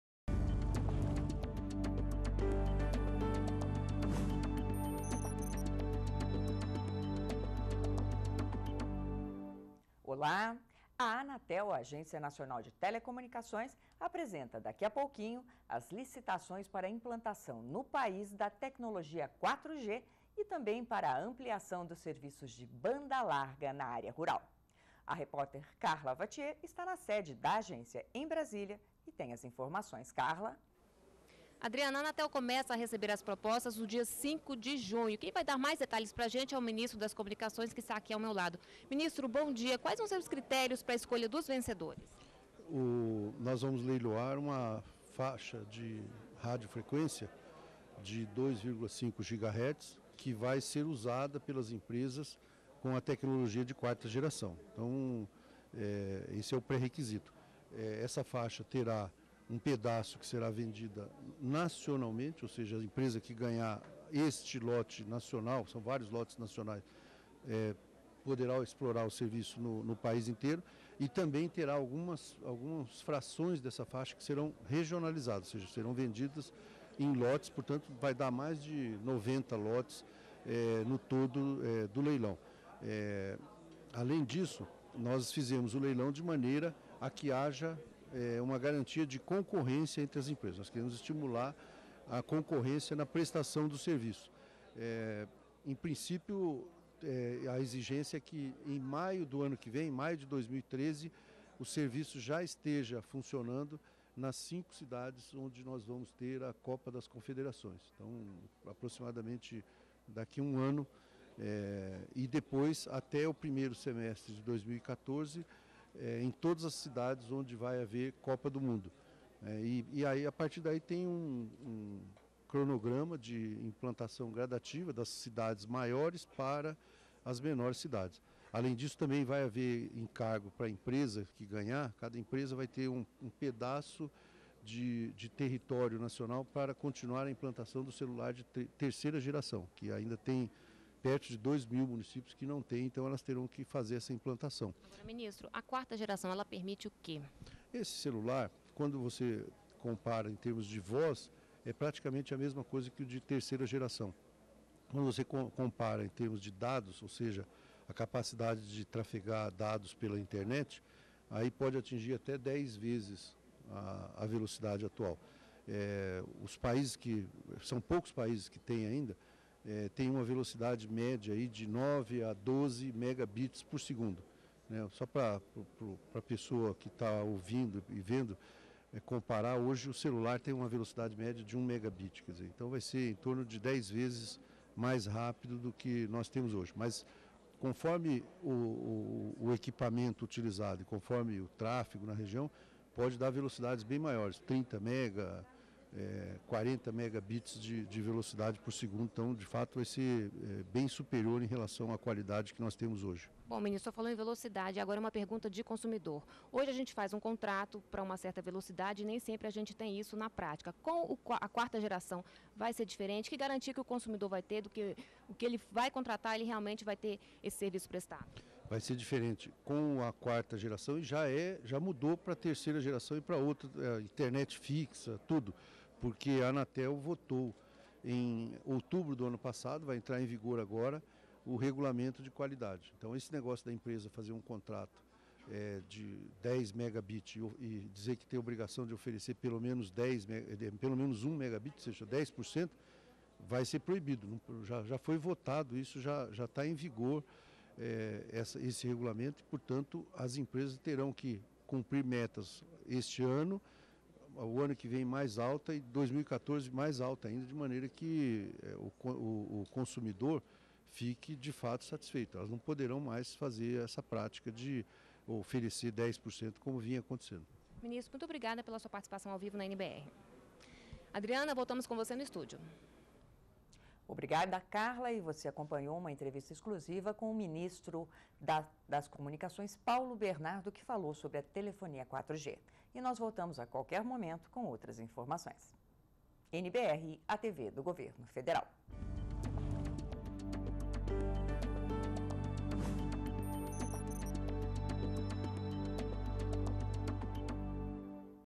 Velocidade da internet 4G é cerca de 10 vezes maior que padrão brasileiro atual
ATIVIDADES DO GOVERNO - 27.04.12: A Agência Nacional de Telecomunicações (Anatel) apresentou nesta sexta-feira (27), em Brasília, as regras de licitação para a implantação da tecnologia 4G, ampliando a cobertura de internet banda larga no país. Antes da cerimônia, a repórter Karla Wathier conversou com o ministro das Comunicações, Paulo Bernardo. Segundo o ministro, o consumidor brasileiro será beneficiado com adoção da nova tecnologia, pois a velocidade da internet banda larga 4G é cerca de 10 vezes mais rápida que as existentes atualmente no país. Para Bernardo, com a internet 4G, o Brasil poderá oferecer um serviço de qualidade à população e aos turistas que visitarão o país na Copa do Mundo 2014 e nas Olimpíadas 2016.